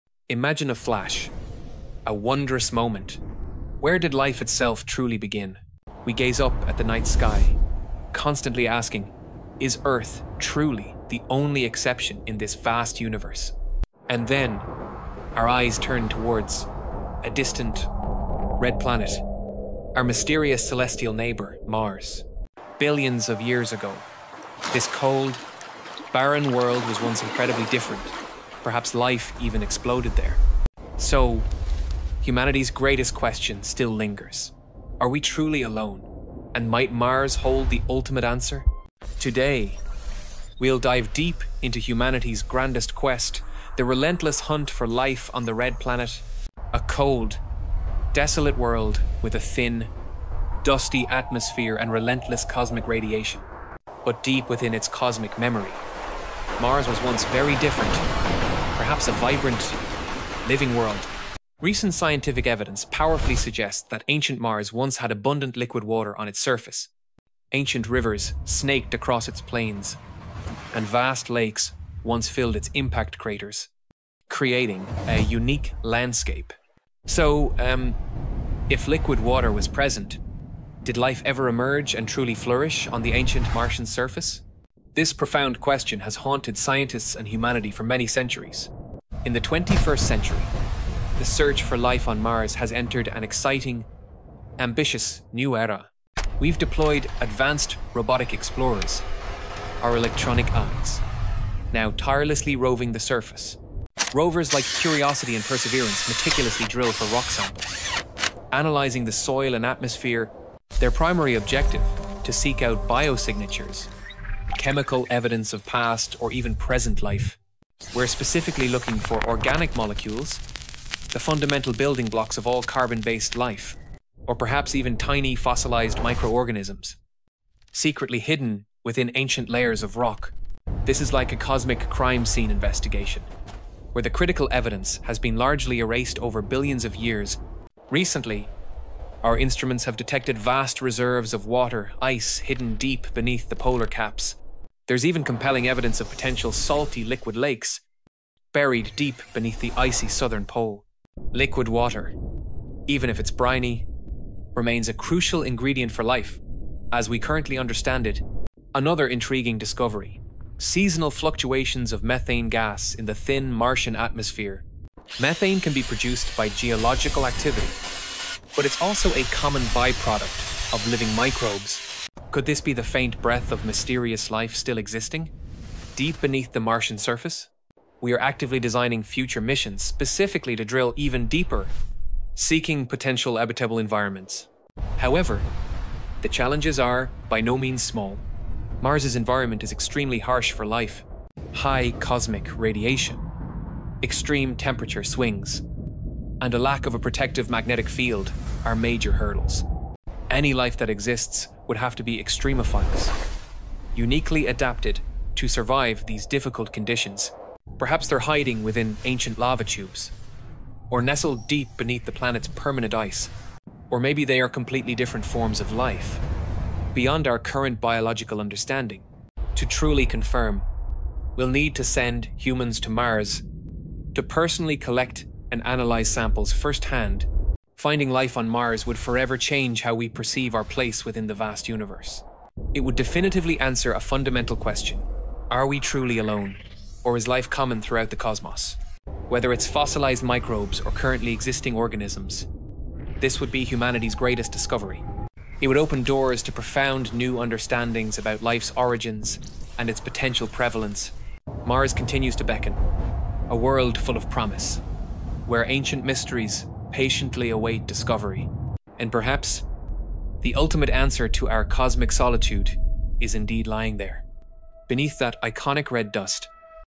Mars: Are We Alone?
Are we alone in the universe? For centuries, humanity has looked to Mars for answers. This video dives deep into the fascinating search for life on the Red Planet, exploring its ancient watery past, the latest rover discoveries, and the intriguing signs of methane in its atmosphere. Join us as we uncover the scientific quest to find extraterrestrial life and what it means for our place in the cosmos.